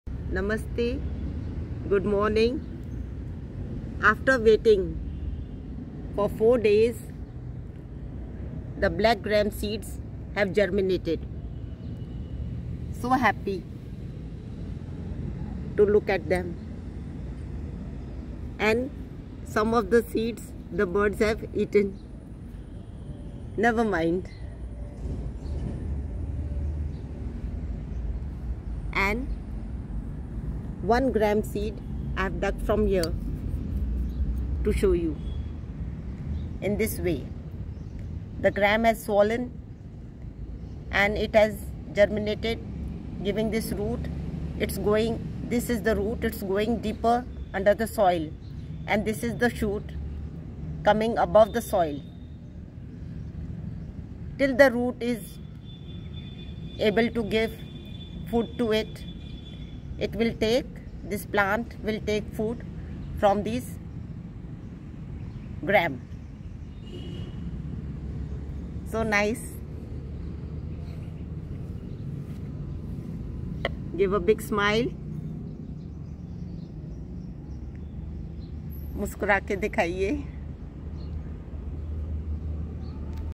Germination of Chickpeas/ Kala Chana ( Part 3 )
Water them only when the soil becomes dry.
They need  less water.